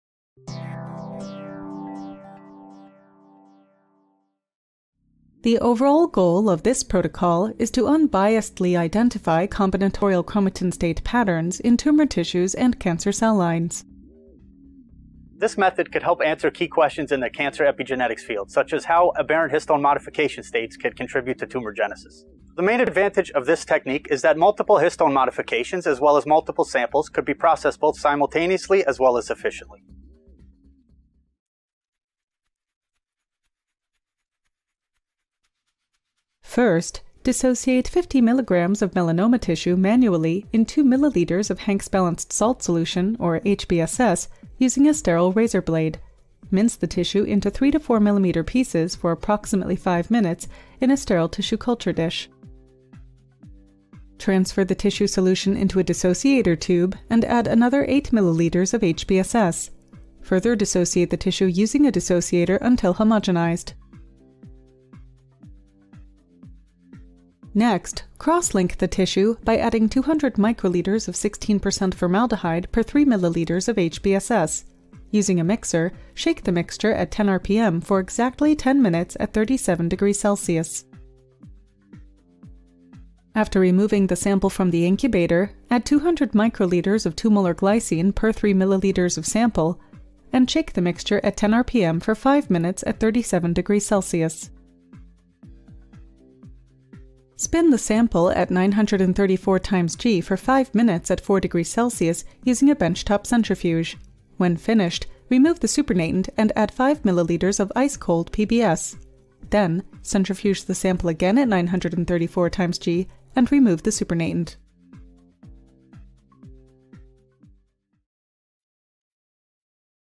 ChIP sequencing in Tumor Tissues for Mapping of Chromatin States| Protocol Preview
An Integrated Platform for Genome-wide Mapping of Chromatin States Using High-throughput ChIP-sequencing in Tumor Tissues -  a 2 minute Preview of the Experimental Protocol

Christopher Terranova, Ming Tang, Elias Orouji, Mayinuer Maitituoheti, Ayush Raman, Samirkumar Amin, Zhiyi Liu, Kunal Rai
University of Texas MD Anderson Cancer Center, Department of Genomic Medicine; The Jackson Laboratory for Genomic Medicine,;

Here, we describe an optimized high-throughput ChIP-sequencing protocol and computational analyses pipeline for the determination of genome-wide chromatin state patterns from frozen tumor tissues and cell lines.